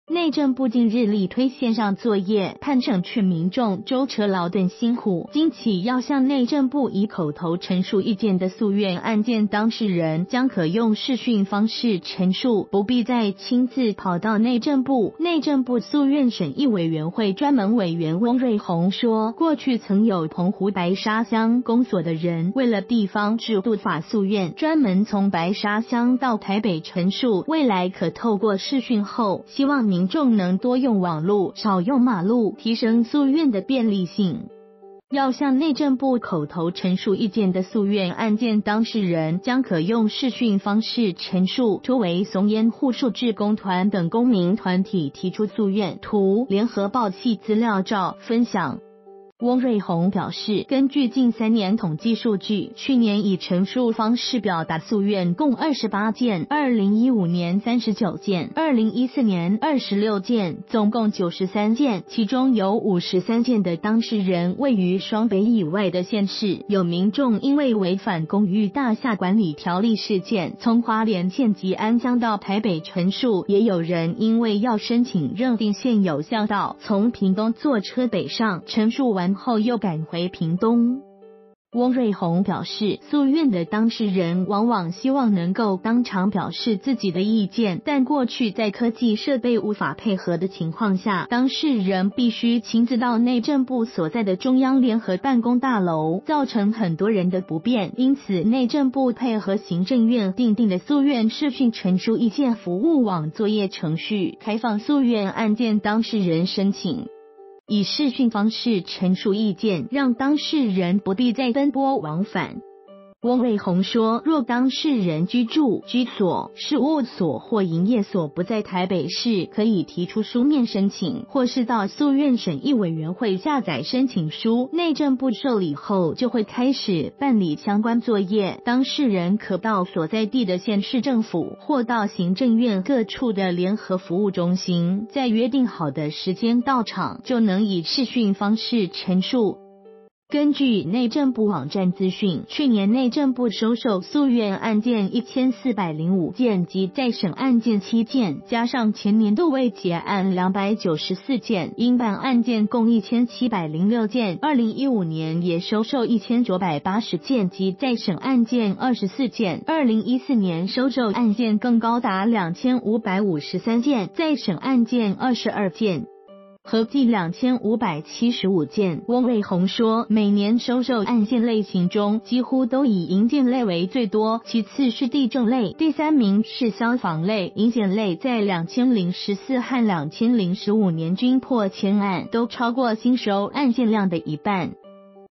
訴願不必奔波了 內政部可用視訊
內政部近日力推線上作業，盼省去民眾舟車勞頓辛苦，今起要向內政部以口頭陳述意見的訴願案件當事人，將可用視訊方式陳述，不必再親自跑到內政部；內政部訴願審議委員會專門委員翁瑞鴻說，過去曾有澎湖白沙鄉公所的人，為了地方制度法訴願，專門從白沙鄉到台北陳述，未來可透過視訊後，希望民眾能「多用網路，少用馬路」，提升訴願的便利性。

發表評論評論如下。